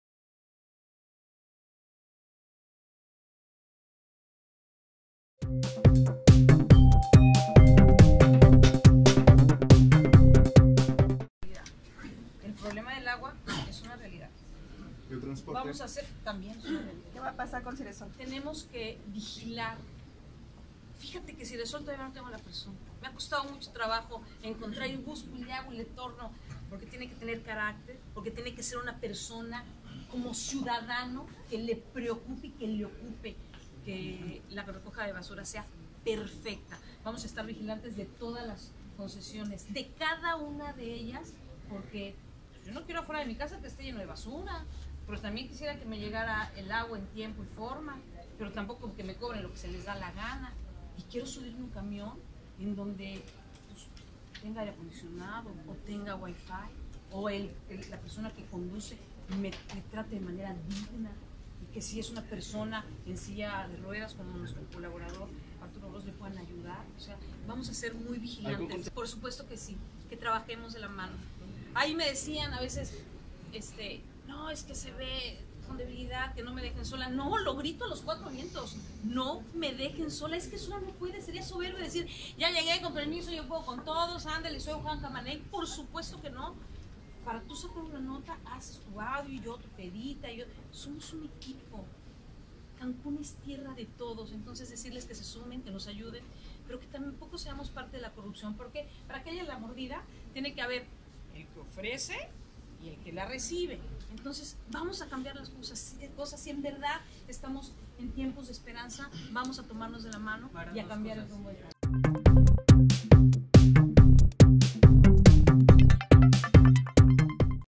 SIRESOL CANCÚN AÚN SIN TITULAR
La presidente Mara Lezama aún no encuentra a la persona idónea para Solución Integral de Residuos Sólidos, Cancún; además reitera que “no la dejen sola” en esta administración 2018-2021.